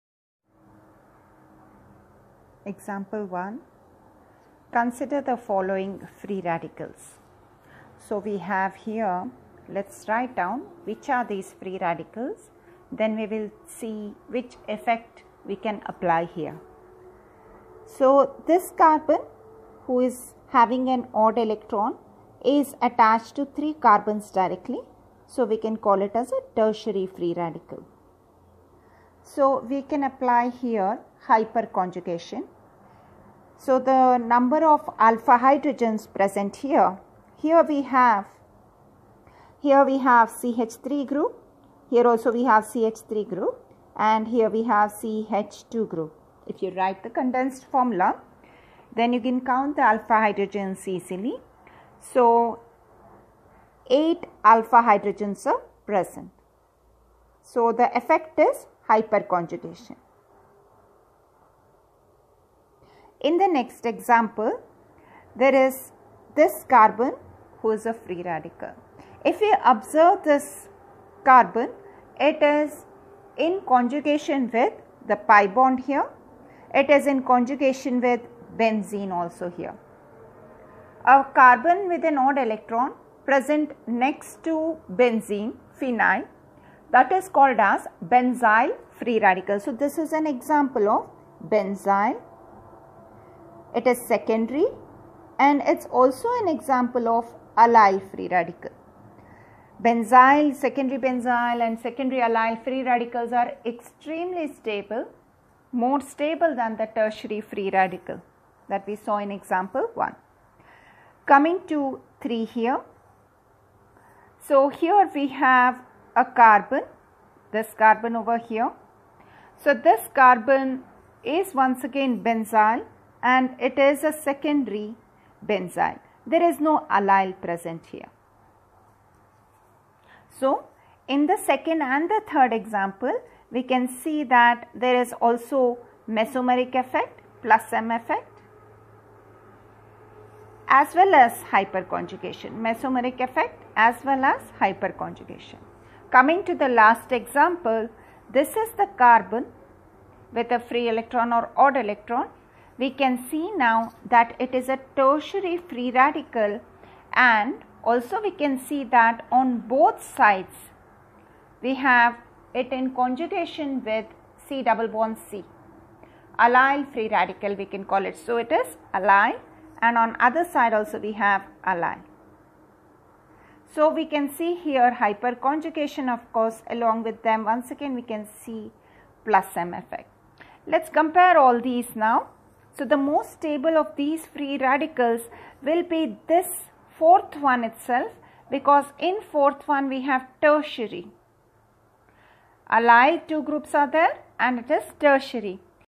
PU 1 | JEE S2 | Chemistry | Organic Chemistry (Basic Principles & Techniques) | Lecture 8 | Slide 26
Welcome to Concept Videos for JEE! This slide will explain Example 1 of Organic Chemistry (Basic Principles and Techniques). We are sure you will have an effective learning experience.